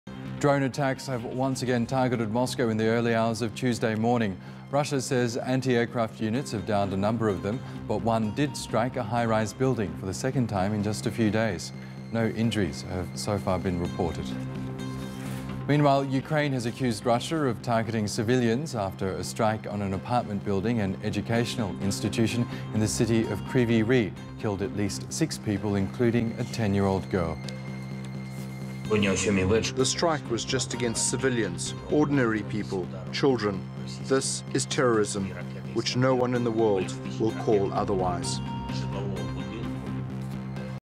Ukrainian drones strike Moscow skyscraper early Tuesday morning
Drone attacks have once again targeted Moscow in the early hours of Tuesday morning. Russia says anti-aircraft units have downed a number of them, but one did strike a high-rise building for the second time in just a few days. No injuries have so far been reported.